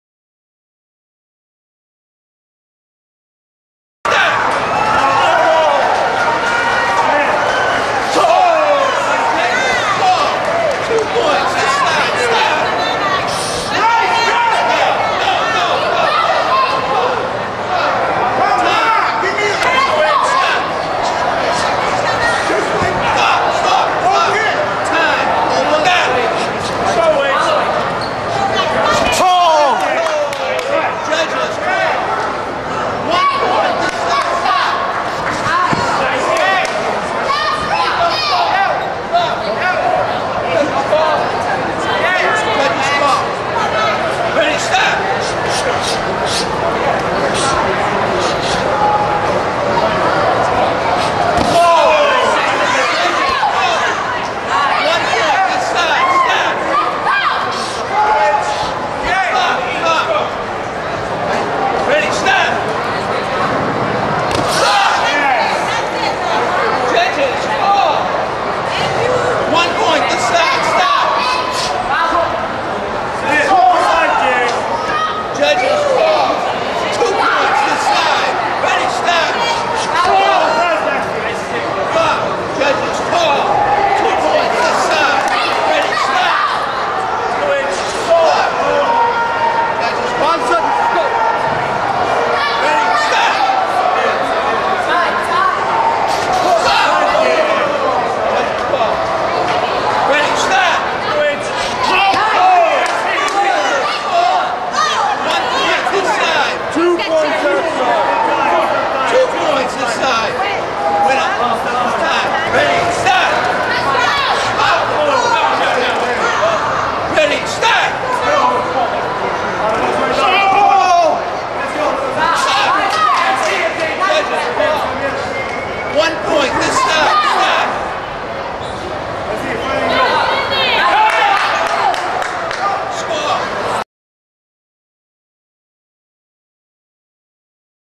Henry Robinson v Maxime Williams - 2007 New England Open
Men's LHW point sparring.
Robinson 7, Wiliams 5.
Division: Point Sparring
Belt Level: Black Belt
Competition Level: Eliminations
Gender: M
Age: Adult

SportMartialArts.com